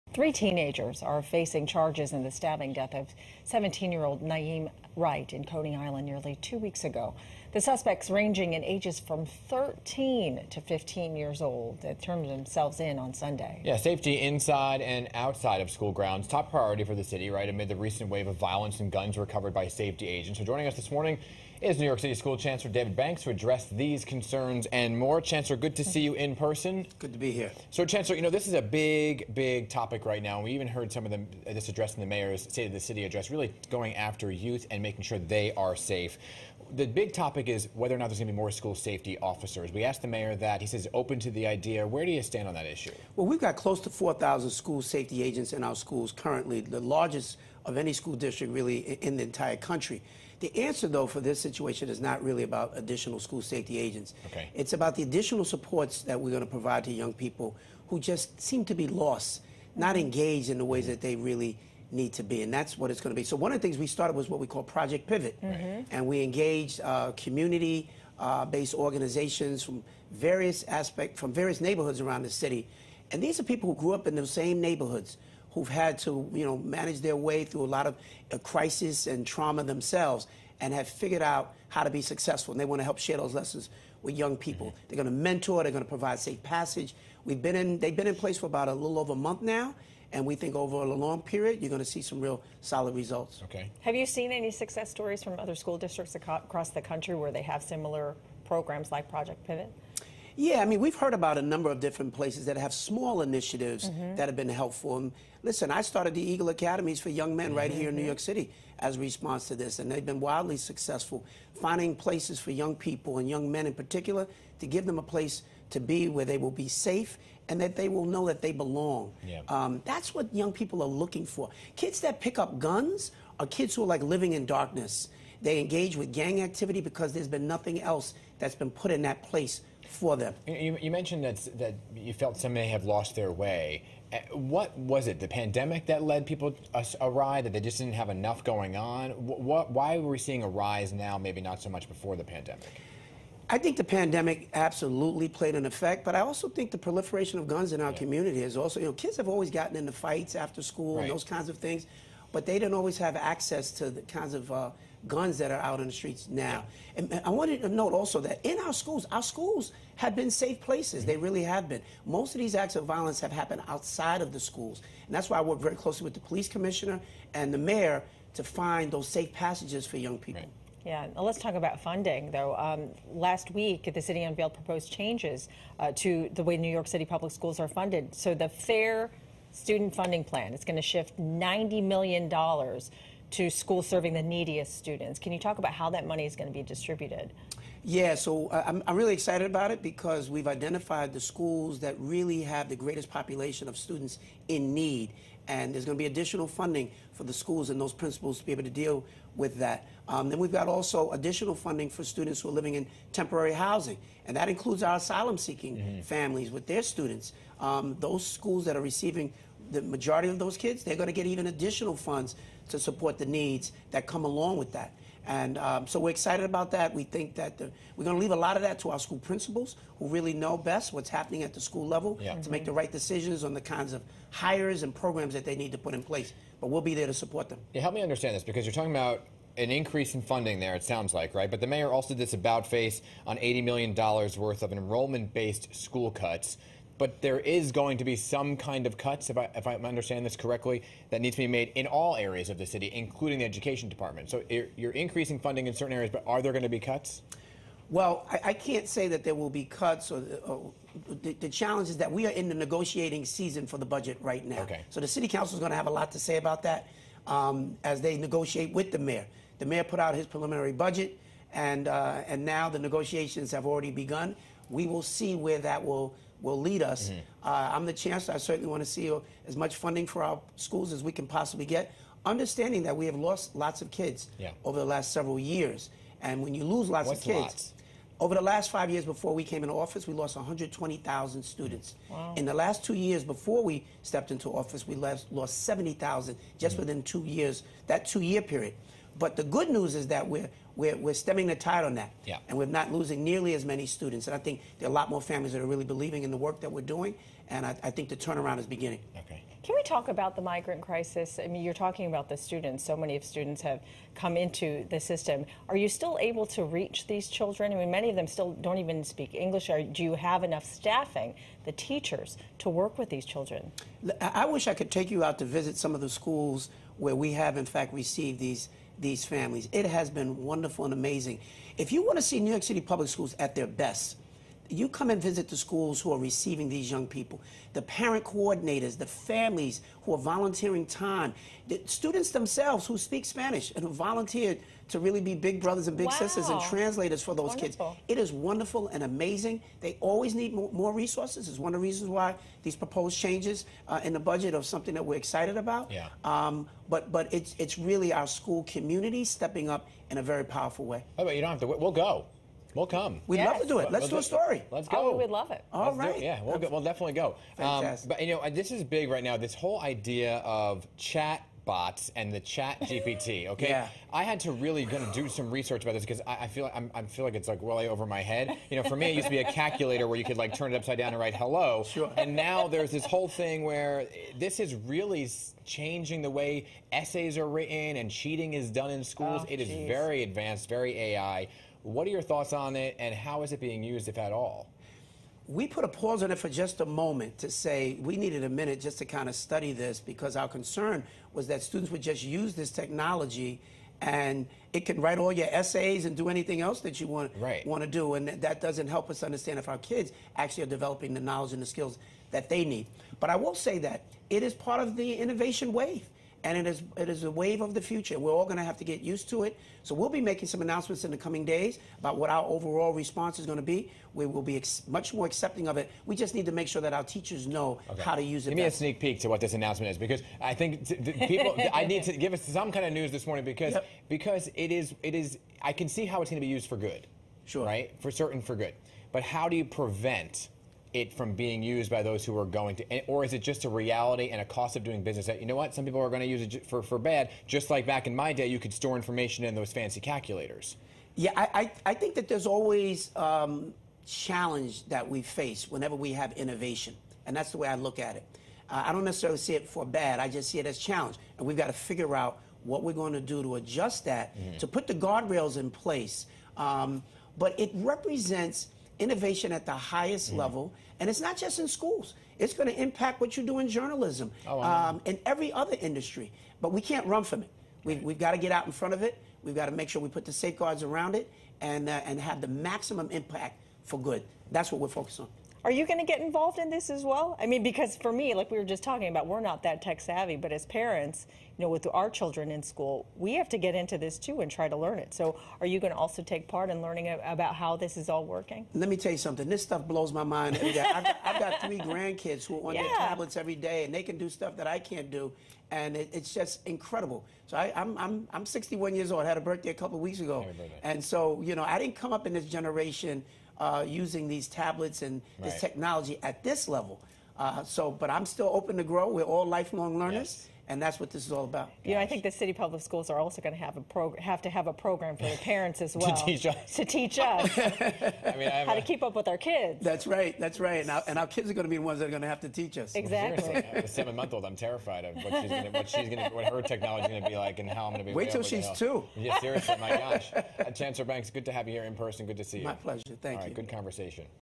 Supports key to keeping NYC kids safe: DOE chancellor
Offering New York City kids better support systems is key to keeping them on the right path and away from the violent crime increasingly touching young lives, Department of Education Chancellor David Banks told PIX11 Morning News on Tuesday.